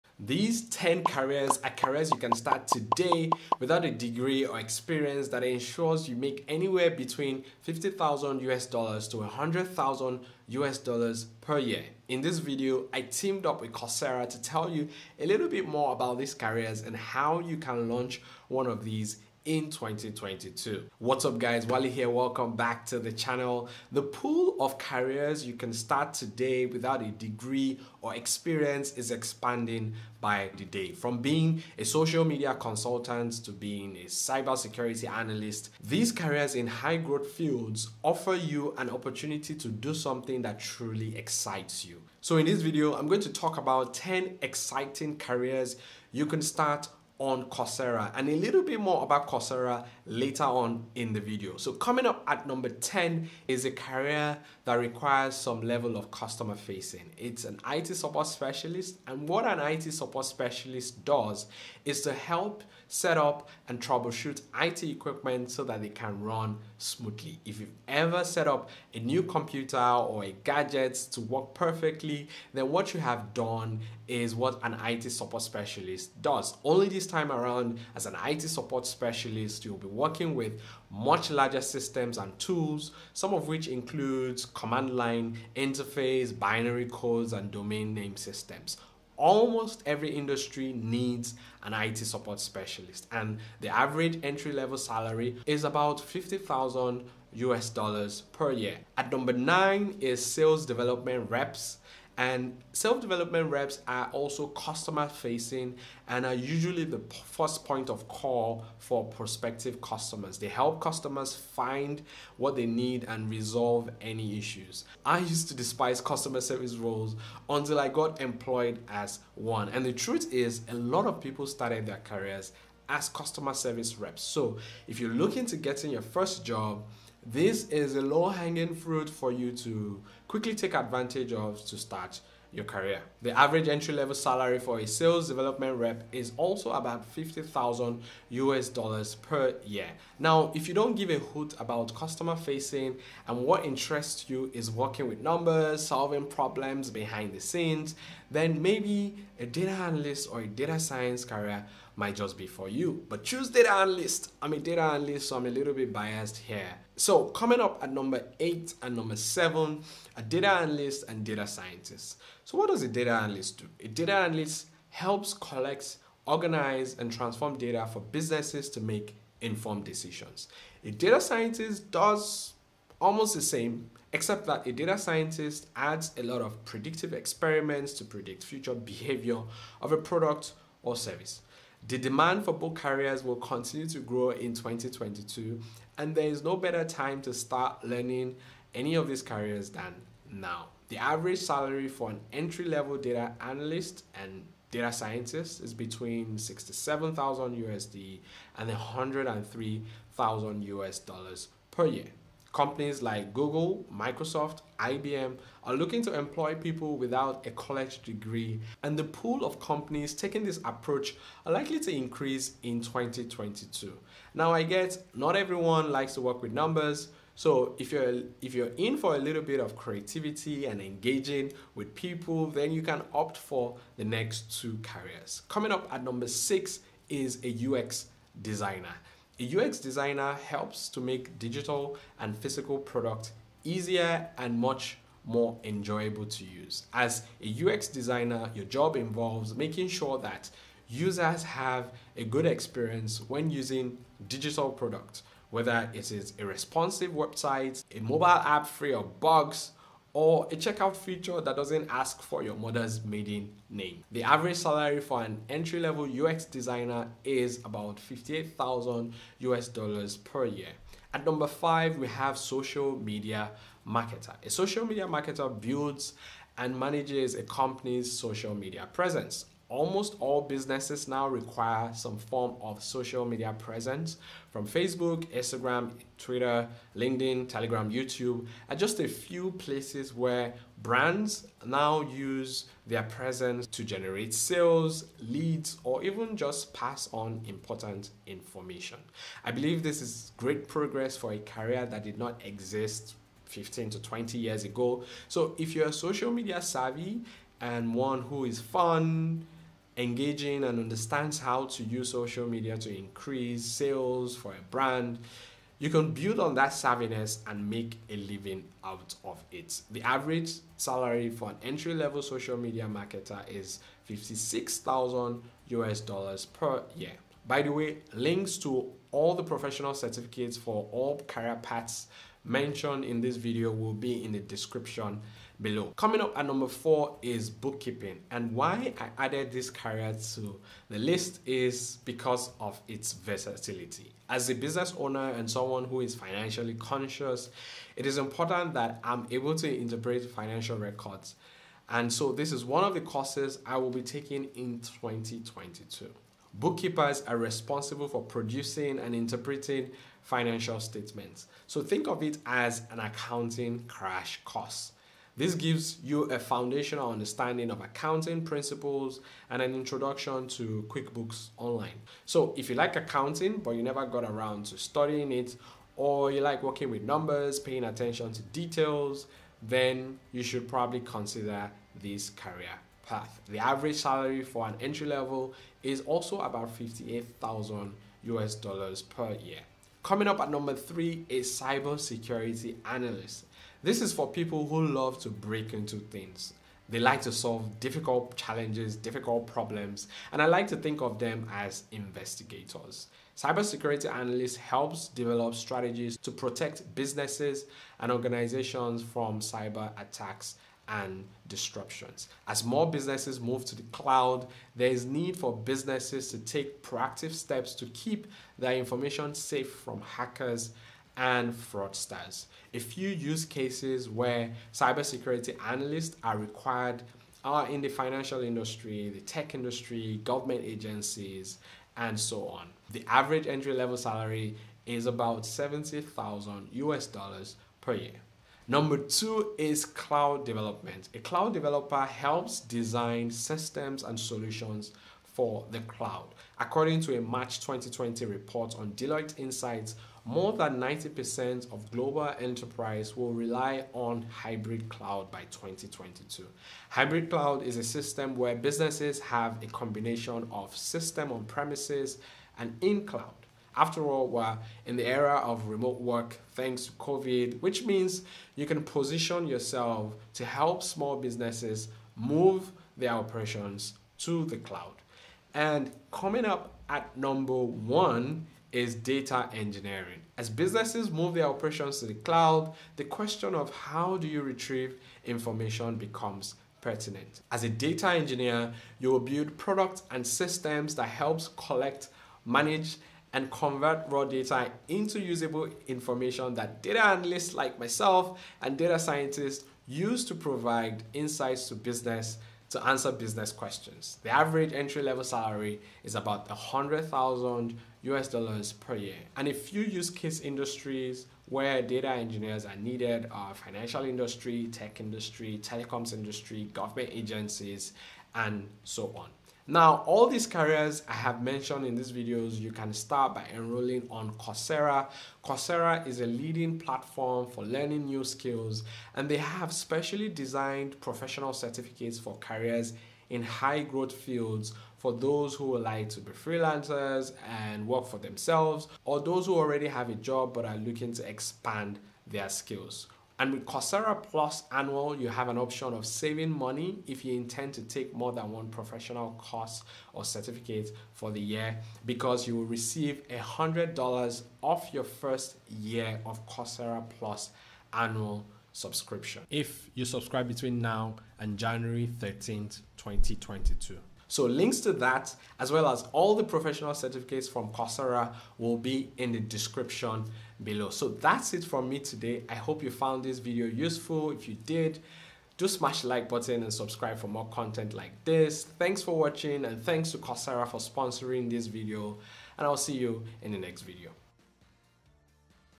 Launch a New Career in 2022 | No Degree or Experience
In this video, I talk about 10 careers you can start on Coursera without need for a degree or experience.

Timecodes
0:00 Intro
0:50 IT Support Specialist
1:39 Sales Development Reps
2:38 Data Scientist & Data Analyst
3:45 UX Designer
4:20 Social Media
5:25 Bookkeeper
6:18 Cybersecurity Analyst
7:07 Cloud Developer
7:46 Data Engineer

Coursera Plus Annual Subscription ($100 off first purchase valid till Jan 13th) Expired

Resources
Edx - Free